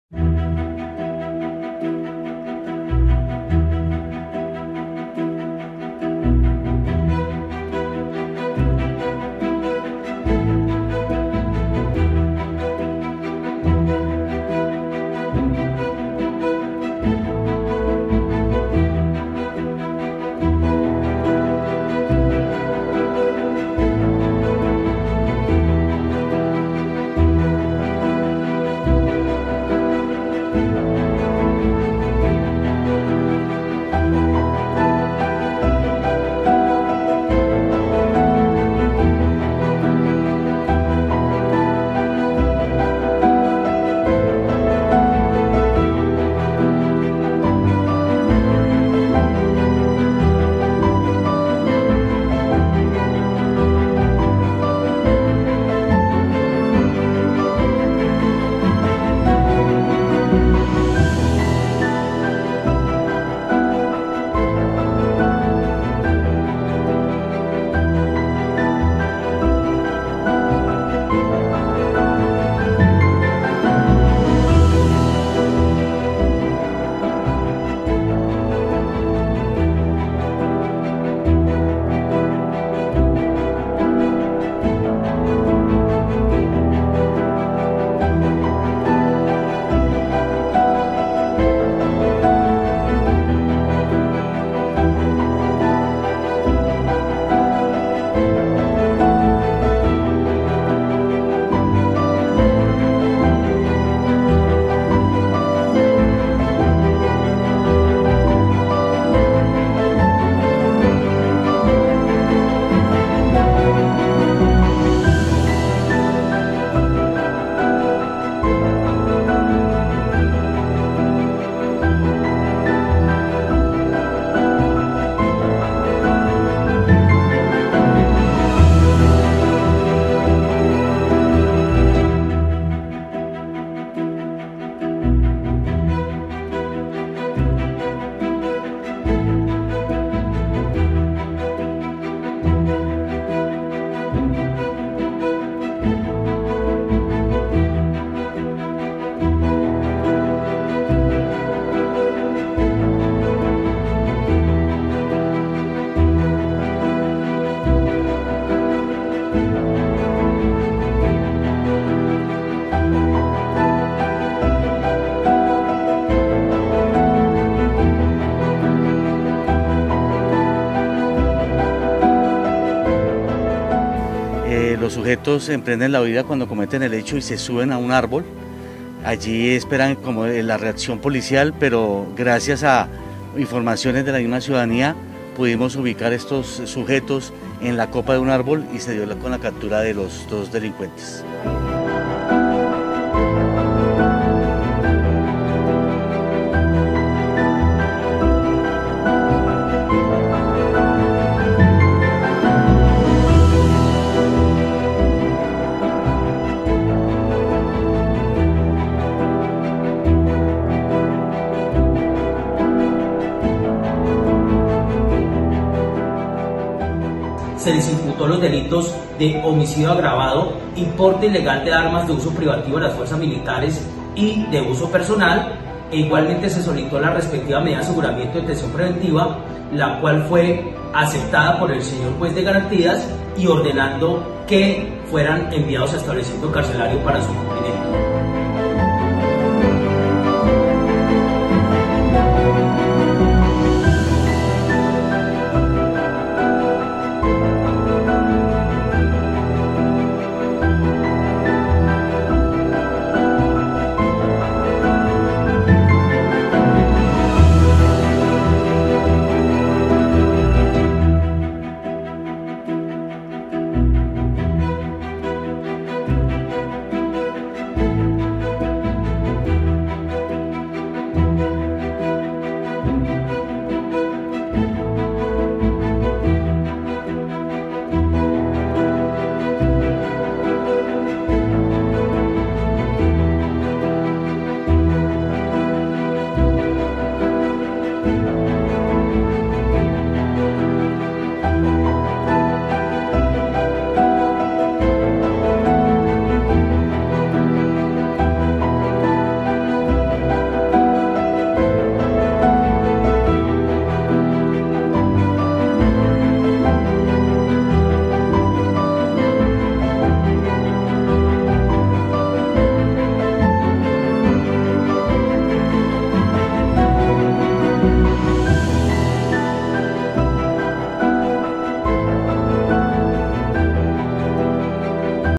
Extorsión por $ 300 millones la hipótesis de la muerte de Junior Jein | El Tiempo
Noticias de último momento: Integrantes de la red criminal 'los Espartanos' habrían ordenado su muerte.